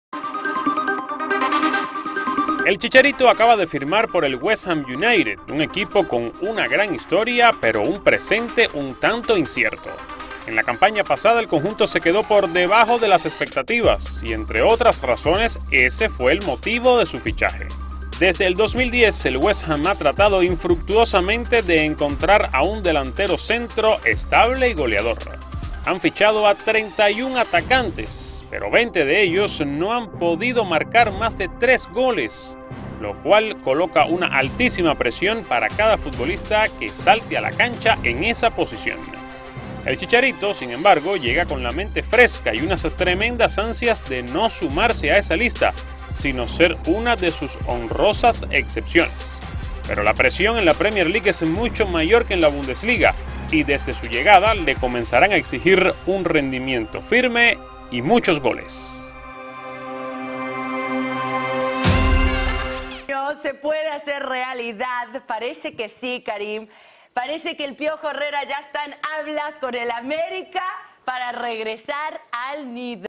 El dato que condena al Chicharito en el West Ham | ¿Sabías qué...? | Telemundo Deportes
Video oficial de Telemundo Deportes. El club apostó por él para romper el maleficio de sus atacantes, pero no le será fácil manejar la presión y romper con esta tendencia.